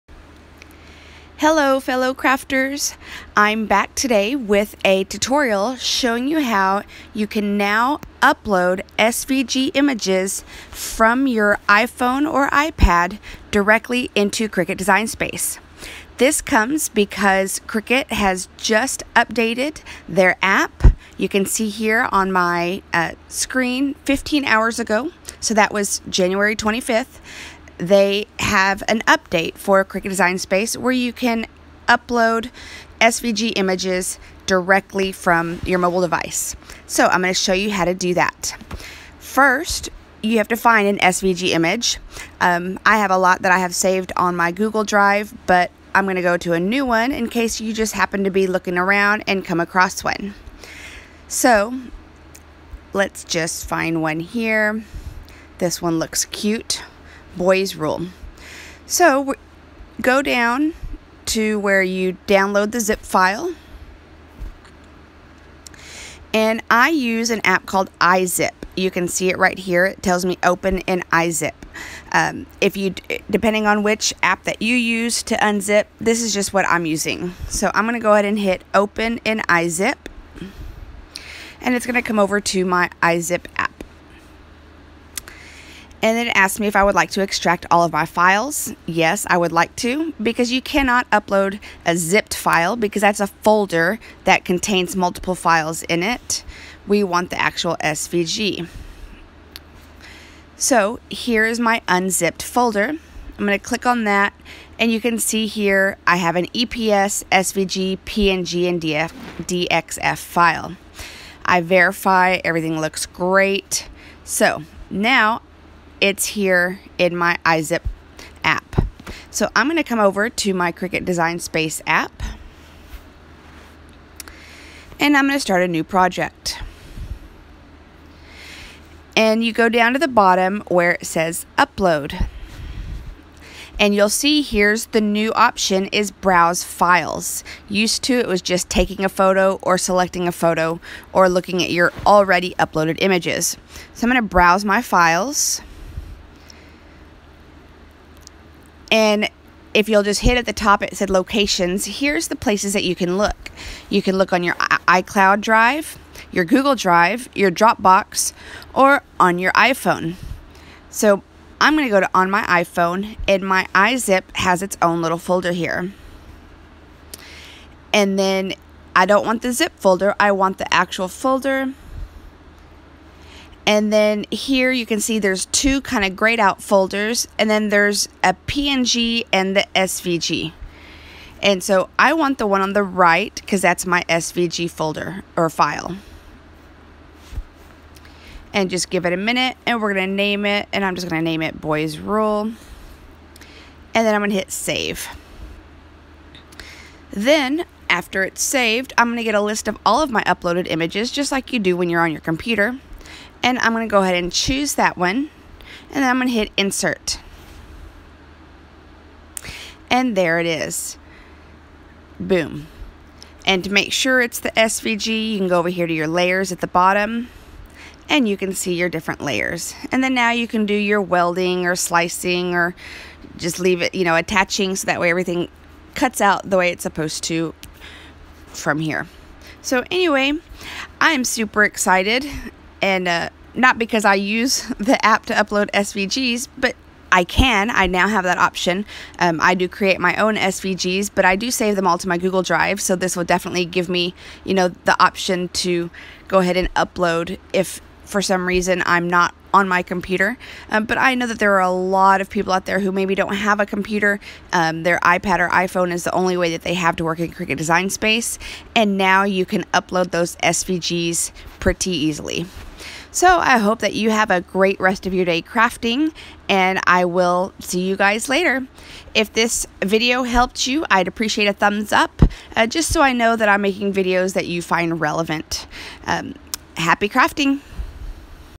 Cricut Design Space - Upload SVG Files on iOS Device + Download & Extract SVG Files
With the January 25, 2018 update, you can now upload SVG files on the CDS app!  I show you how you can download a ZIP file that contains an SVG, extract the files, and then upload them into the app.  You can also upload from your Google Drive, Dropbox, or iCloud Drive!!  If you found this helpful, please don’t forget to like it!!